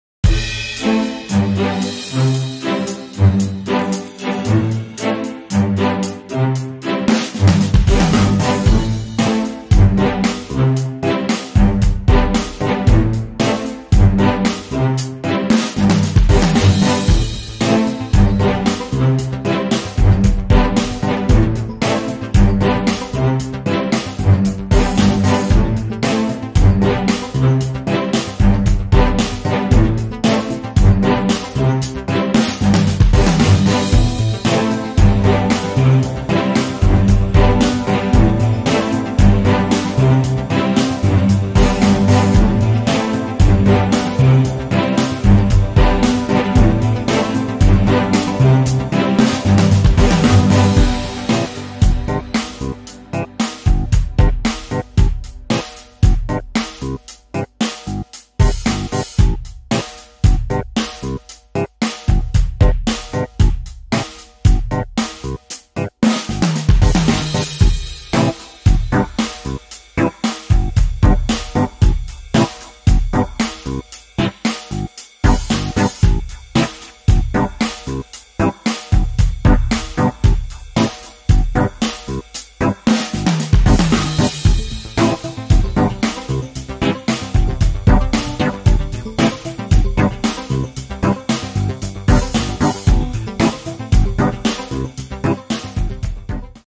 Complete filling line for chemical products HEMILINE 40
Fillind and Capping line for chemical products.